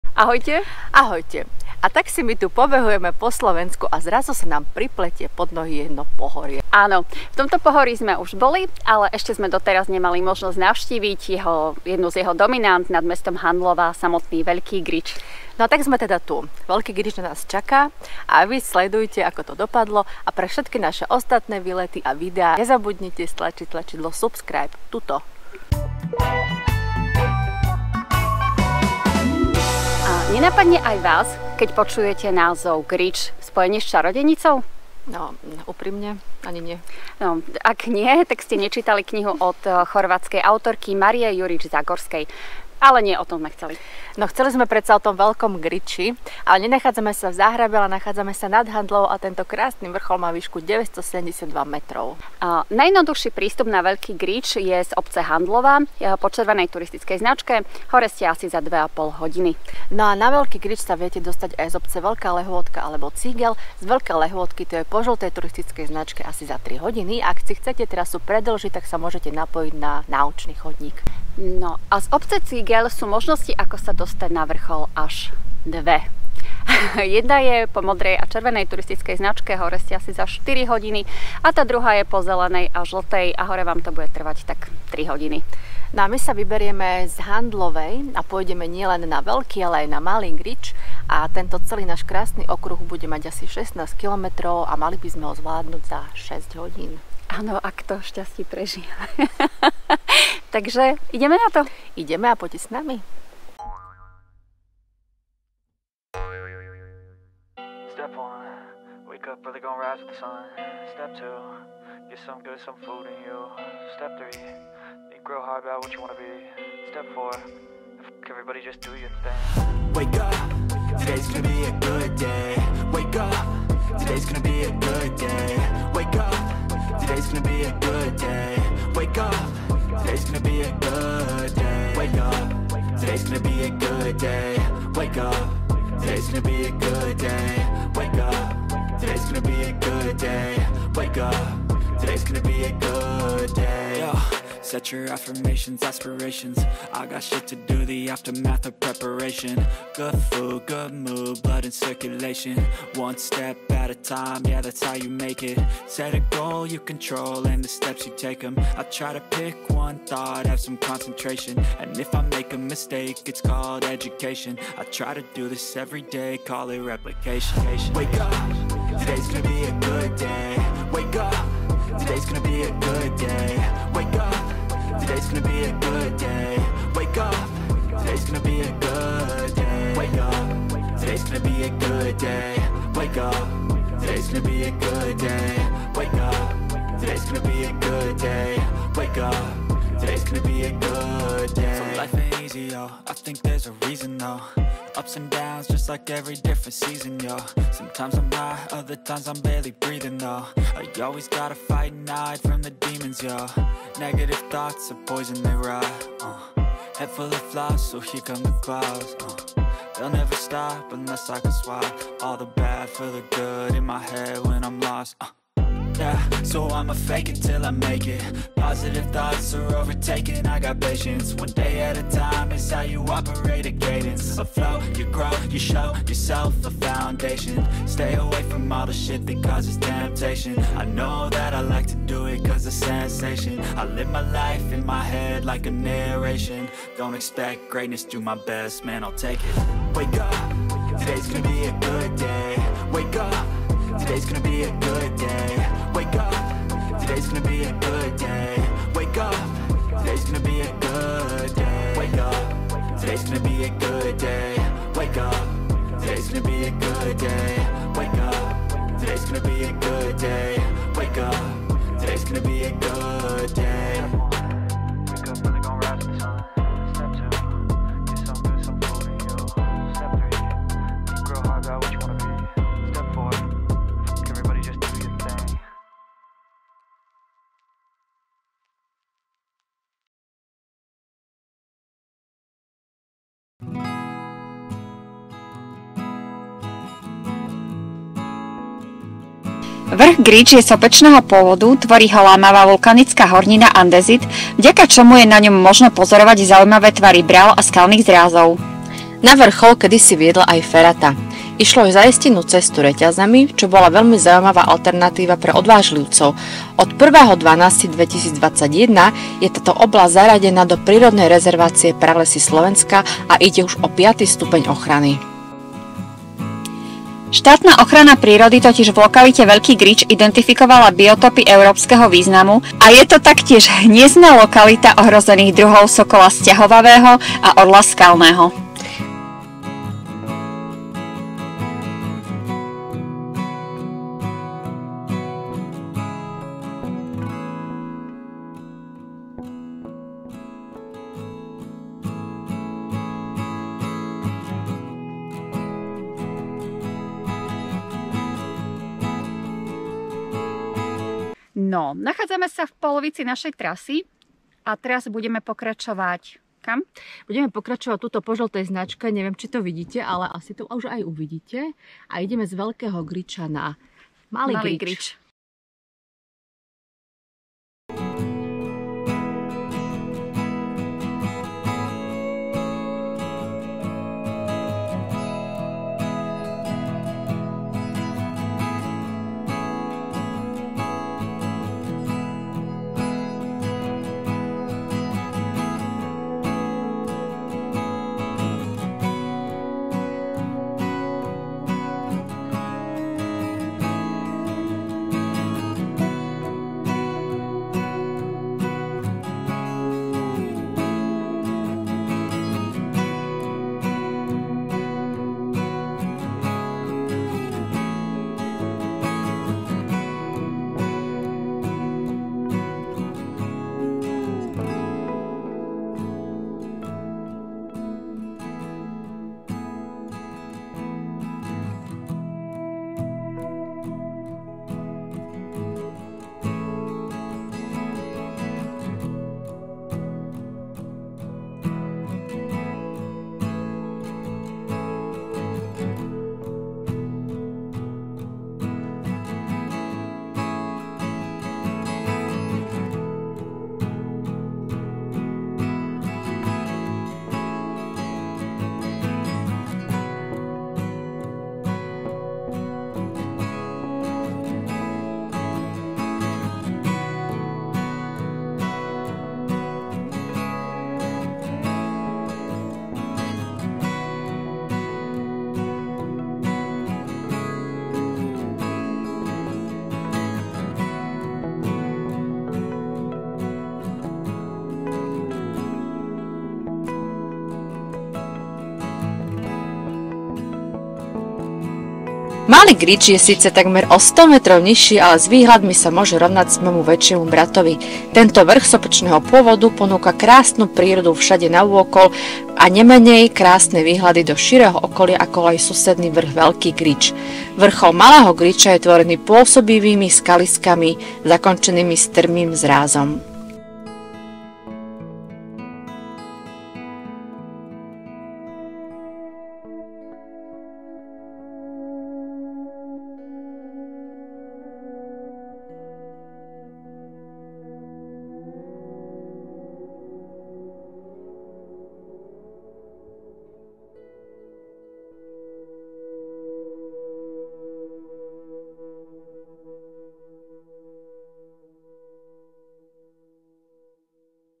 Veľký a Malý Grič |Vtáčnik | Handlová | Turistika | VaGa
Výlet na Veľký Grič a odtiaľ na Malý bol skutočne nádherný.
Nielen, že nám vyšlo počasie, ale trasa ponúkala rozmanitosť, krásne výhľady a len trošku stúpania.
Ak sa vyberiete po tej istej trase ako my, a z Veľkého  Griča budete chcieť ísť na Malý, dajte si pozor na odbočku zo žltej na zelenú. Zelenou traou akoby nikto nechodil a z cesty sme pár krát zišli. Zo zelenej odbočíte na väčšej lúke do prava a dostanete sa opäť na značku žltú, ktorá vás dovedie na vrchol Malého Griča.
Nič vás na odbočku neupozorní, tak si pre istotu zapnite navigáciu :)

Ak by ste chceli ísť s nami na kemp, nezabudnite si rezervovať miesto na